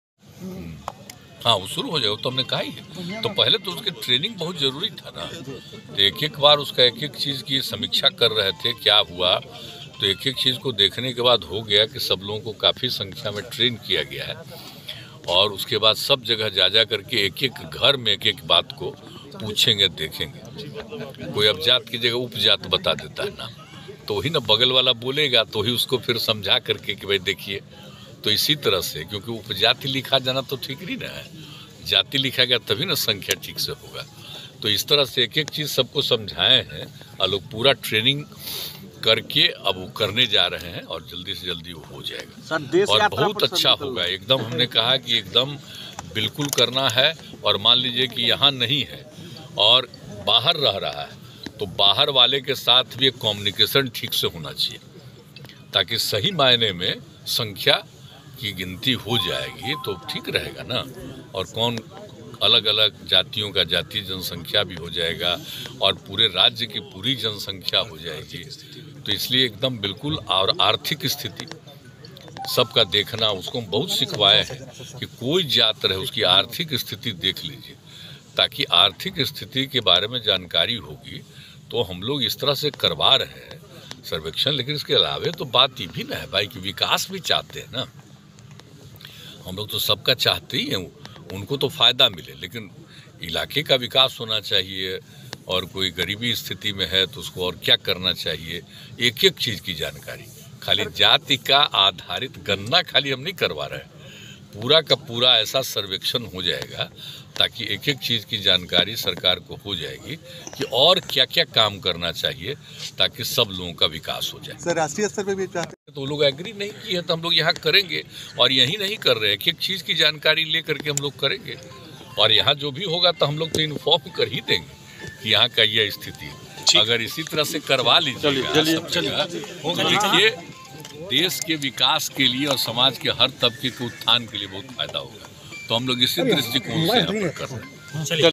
Biharजाति आधारित जनगणना से बिहार वासियों को बहुत फायदा होगा
मुख्यमंत्री नीतीश कुमार ने कहा कि जातिगत जनगणना से बिहार के लोगों को कई प्रकार के फायदा होंगे घर घर जाकर कर्मियों के द्वारा जाति के साथ साथ उनकी प्रस्तुतियों के बारे में भी जानकारी ली जाएगी इससे उन्हें काफी फायदा होगा और सरकार को यह भी पता चलेगा कि कितने लोग बेरोजगार हैं और कौन से लोग किस व्यवसाय से जुड़े हुए हैं और कितने लोग असहाय हैं कितने लोग गरीब हैं इन सभी प्रकार से लोगों को जाति के आधार पर सहूलियत दी जाएगी और हर तबके के लोगों को सहूलियत दी जाने की बात कही जा रही है और सभी को बेहतर से बेहतर सुख सुविधाएं भी उपलब्ध कराए जाने की व्यवस्था की जाएगी
# जातिगत जनगणना बिहार 2023
न्यूज़ बिहार नेशन
मुख्यमंत्री नीतीश कुमार
विश्वनाथ प्रताप यादव अरवल न्यूज़